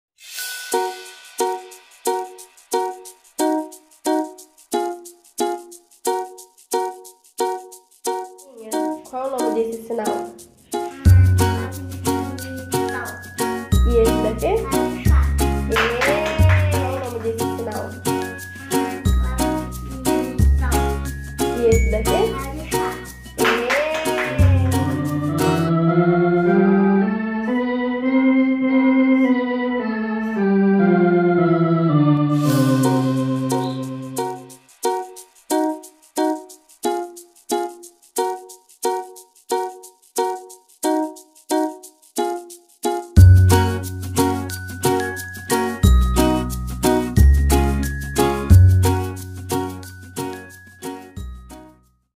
Representação das Clave de Sol e Fa para tocar os métodos e hinário
Profa Sandra / Hortolândia SP.
Ensinando crianças com 4 e 6 anos de idade a representação das claves para tocar as lições dos métodos e também futuramente o hinário.